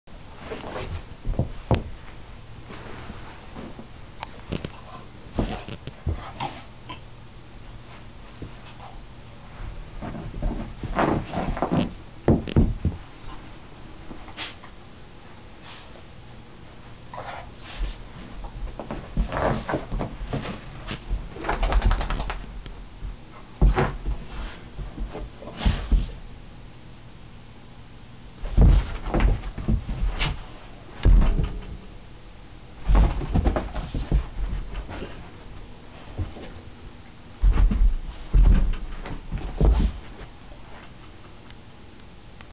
My dog doing stretching exercise
He is doing a stretching exercise before taking a walk and then  ready to go!